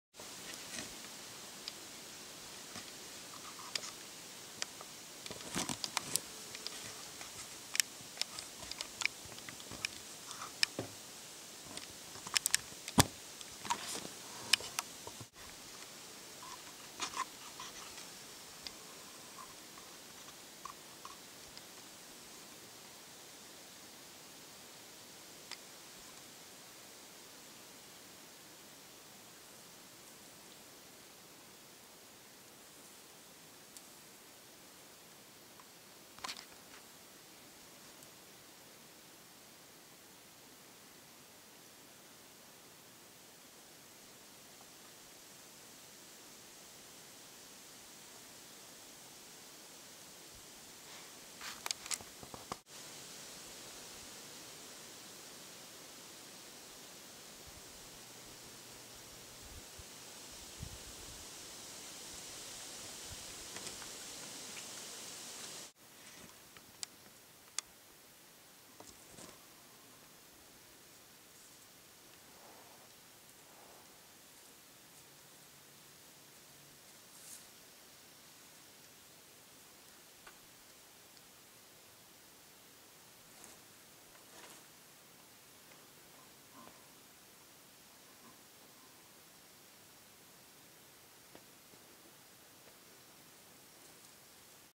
Minnesota Bear Hunting 2014
First day bears with Tim Humphrey from Aspen Outfitters.